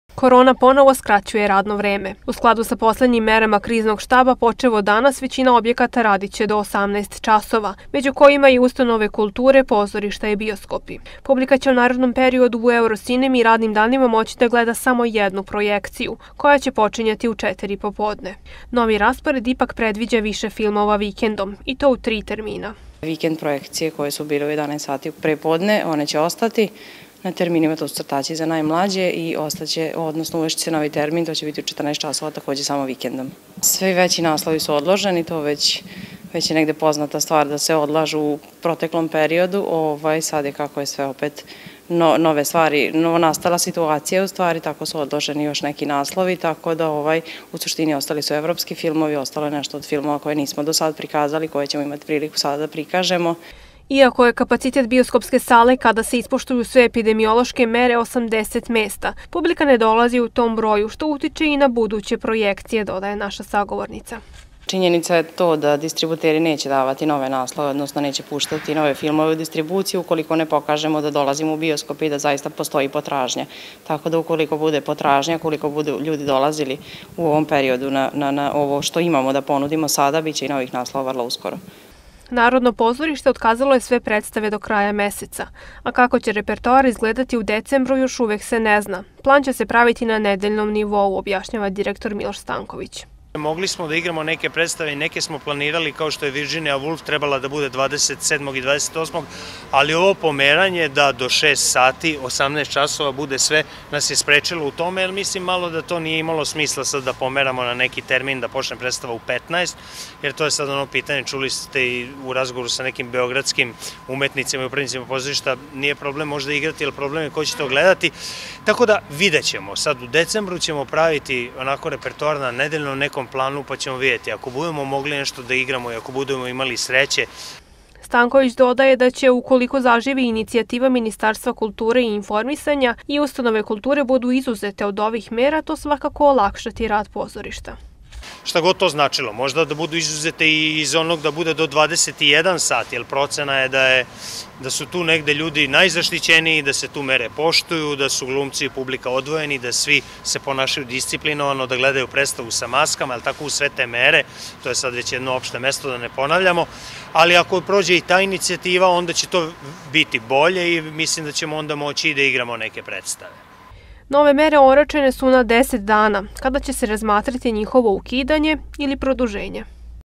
U pozorište i bioskop do 18 časova
Korona ponovo skraćuje radno vreme. U skladu sa poslednjim merama Kriznog štaba  počev od danas većina objekata radiće do 18 časova, među kojima i ustanove kulture, pozorišta i bioskopi. Publika  će u narednom periodu u Eurocinemi radnim danima moći da gleda samo jednu projekciju, koja će počinjati u  četiri popodne. Novi raspored ipak predviđa više filmova vikendom, i to u tri termina.

Iako je kapacitet bioskopske sale kada se ispoštuju sve epidemiološke mere 80 mesta, publika ne dolazi u tom broju, što utiče i na buduće projekcije, dodaje naša sagovornica.

Narodno pozorište otkazalo je sve predstave do kraja meseca, a kako će repertoar izgledati u decembru još uvek se ne zna, plan će se praviti na nedeljnom nivou, objašnjava direkor Miloš Stanković.

Stanković dodaje da će ukoliko zaživi inicijativa Ministarstva kulture i informisanja i ustanove kulture budu izuzete od ovih mera, to svakako olakšati rad pozorišta.

Nove mere oročene su na deset dana, kada će se razmatrati njihovo ukidanje ili produženje.